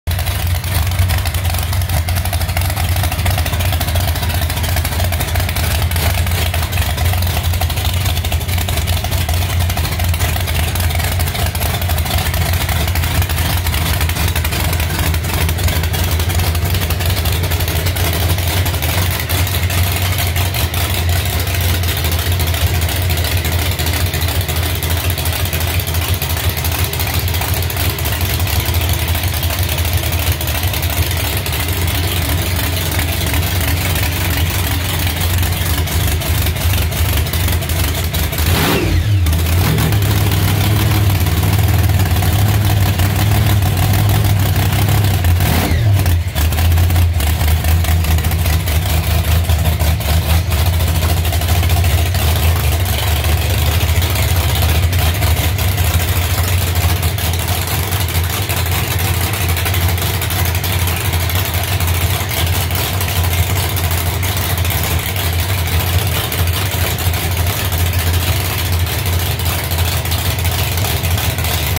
CoupeZilla first start-up caught idling in his crib.
Decided to let him idle awhile and circulate some nice clean fuel through his veins.